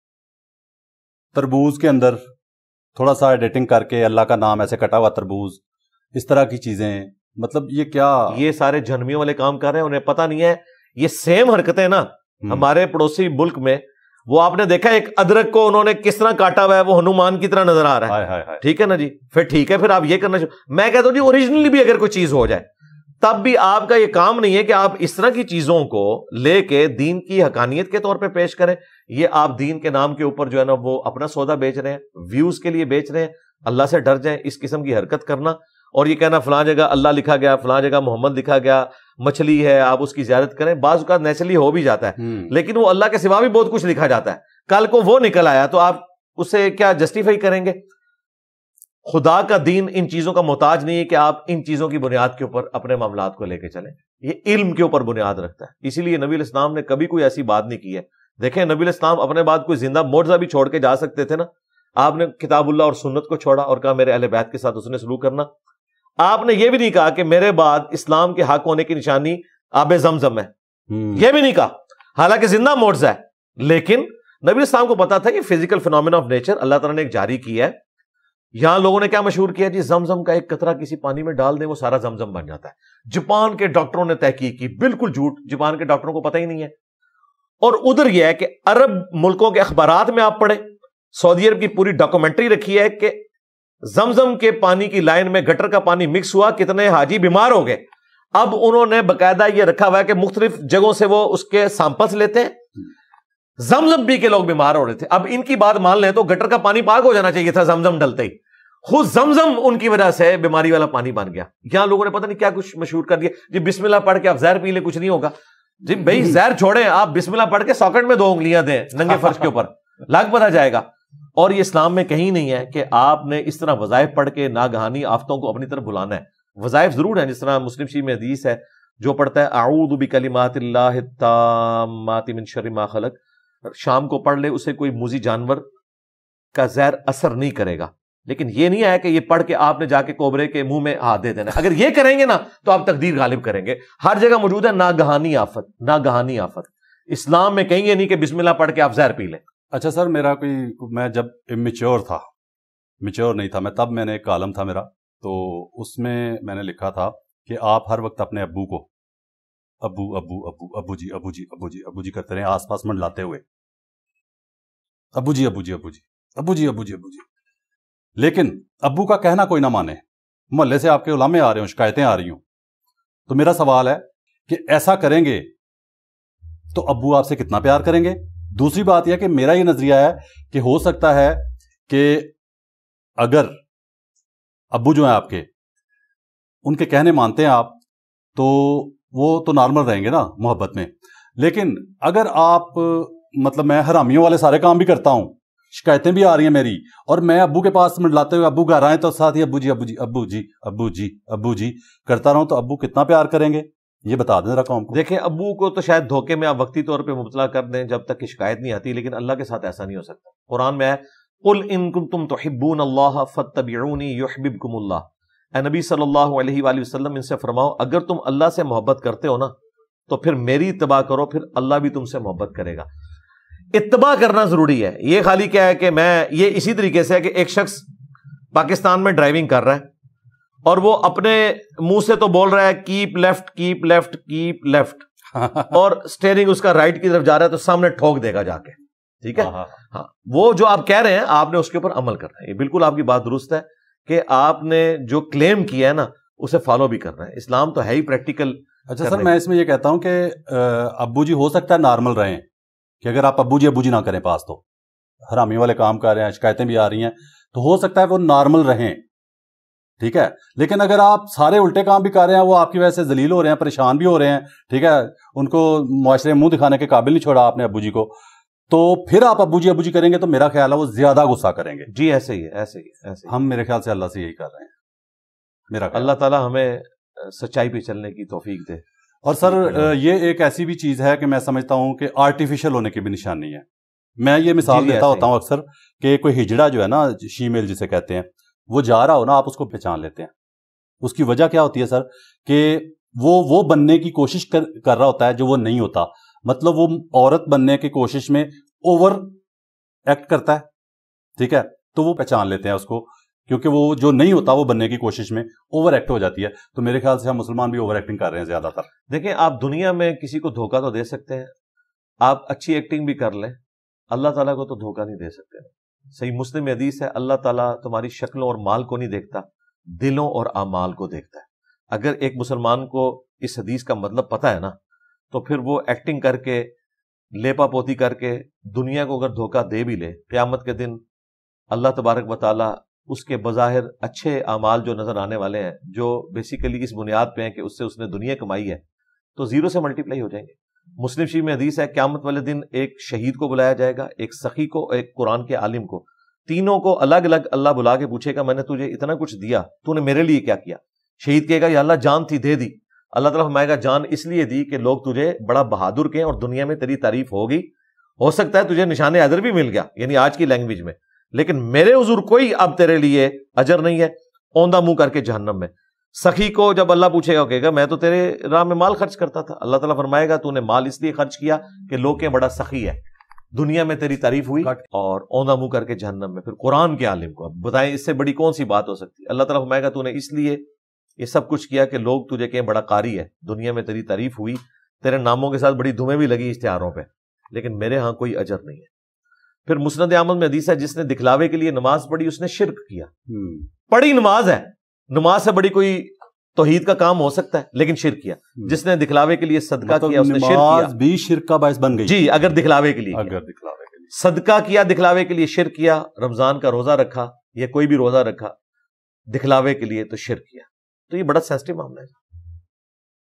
😍 Religious Symbols In Fruits & Vegetables | Sirf ALLAH ALLAH Karna | Engineer Muhammad Ali Mirza
Qur'an & Sunnat Research Academy Jhelum, 2nd Floor in Mahfooz Plaza, Opposite Pak Army Recruitment Office Jhelum.

(9am to 5pm Except Sundays)

Engr. Muhammad Ali Mirza (Complete Lectures Channel)